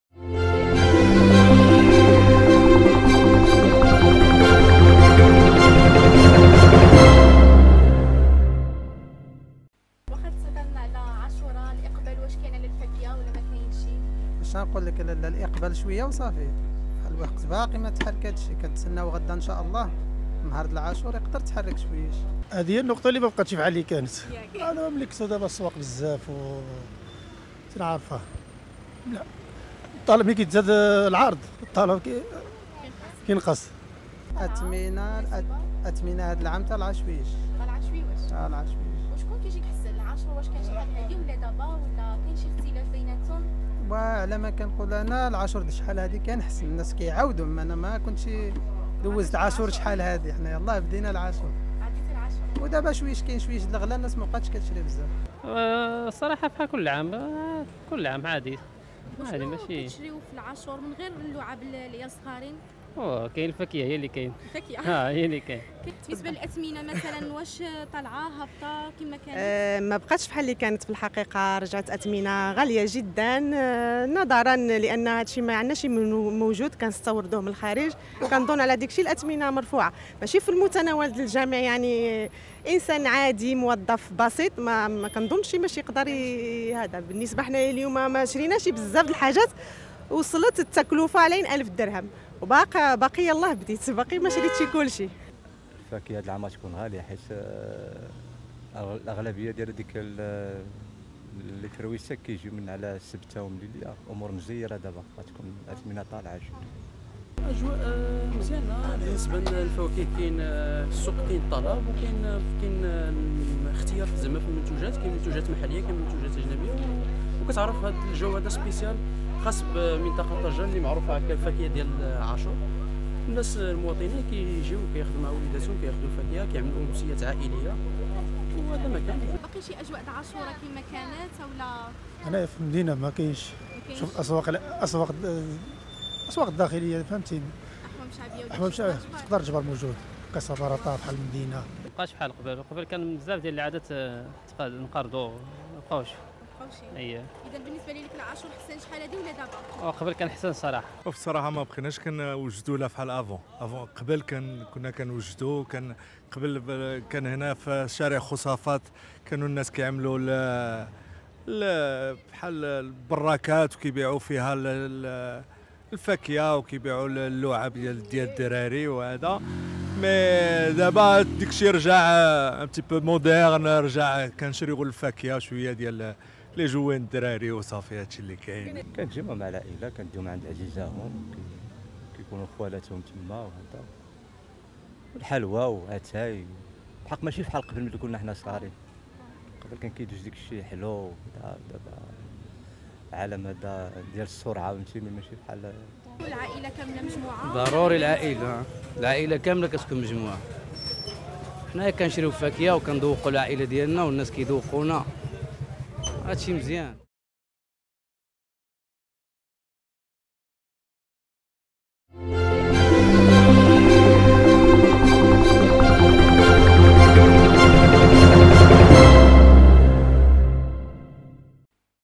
هذه أجواء الاحتفال بعاشوراء بطنجة
الاحتفاء بعاشوراء بات تقليدا سنويا لدى المغاربة، الكل له طريقته الخاصة في الاحتفال، لكن أغلب من استقى موقع "طنجاوي" آرائهم حول هذا اليوم، يجمعون على أن هذه مناسبة يجتمع فيها الأهل والعائلة والأحباب، يتبادلون الزيارات، ويقتنون "الفاكية"، مسمتعين بيومهم البهيج.
 التجار أيضا يعدون محلاتهم، التي يرتادوها الزبناء لاقتناء اللعب  وغيرها من الهدايا التي  تقدم للأطفال بمناسبة يوم عاشوراء.
موقع "طنجاوي" خرج اليوم إلى الشارع وأعد لكم هذا الروبورتاج حول  سوم عاشوراء..